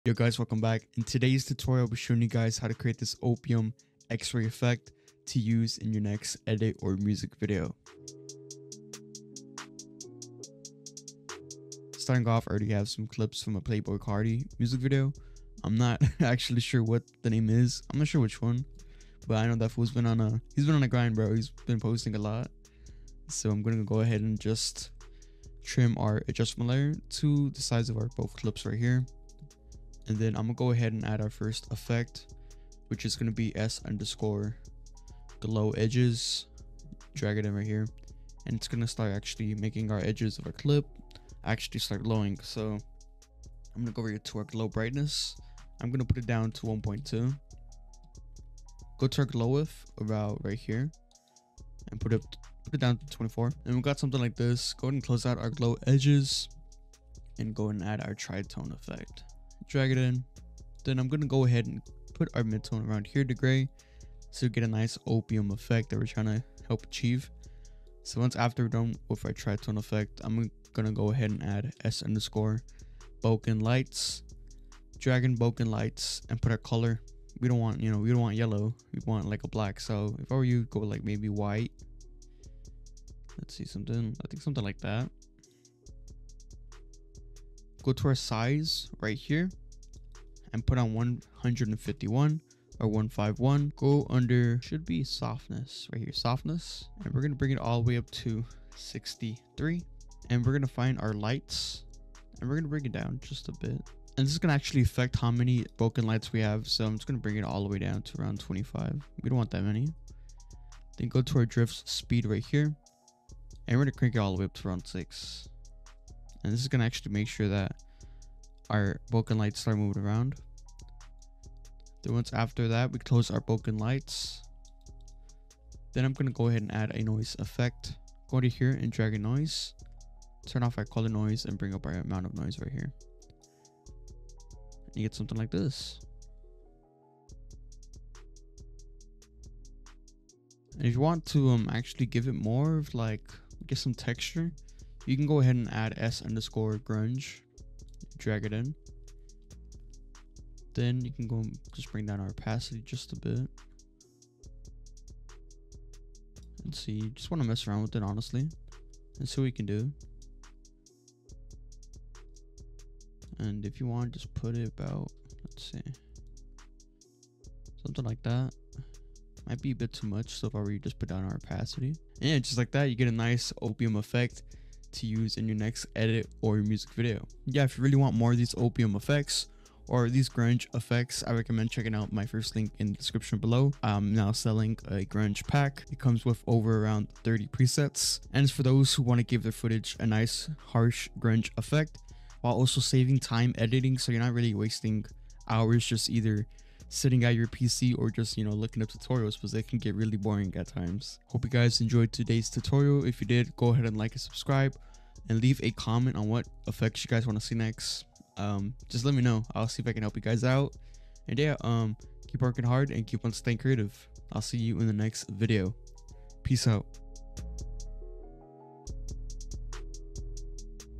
Unique OPIUM Effect! (After Effects Tutorial)
In this easy tutorial, I’ll walk you through how to create that dark, grungy vibe you see in the underground scene. Perfect for anyone looking to level up their edits and add a unique aesthetic to their projects.